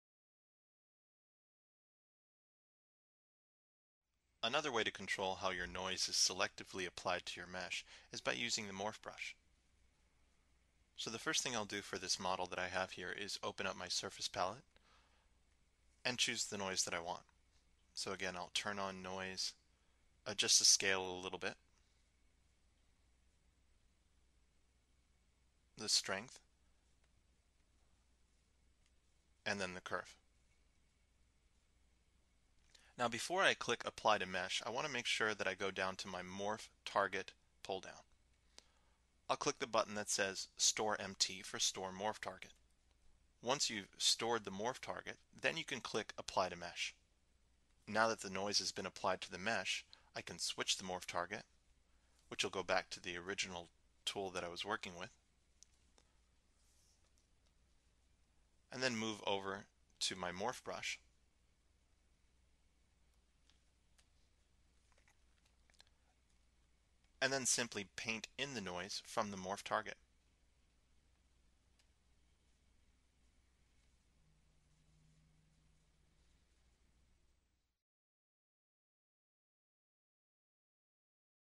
ZBrush Tutorials (Surface Noise) - Noise nad the Morph Brush
Introduced in ZBrush 3.5 is the new surface noise. These videos will take you through the basics of applying noise to surface, brushes, polypainting, and masking.